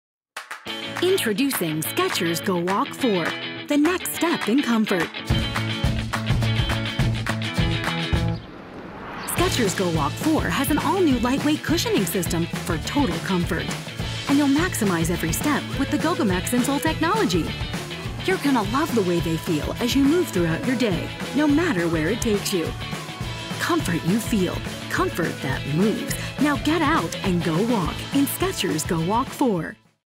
Skechers Go Walk 4 Kindle | Women's Walking Shoe | Intersport Elverys
The Skechers GOWalk 4 Kindle Shoe features a 5GEN® midsole and a seamless one piece upper with the new Gogo Max™ insole, walking will never be the same once you try these on.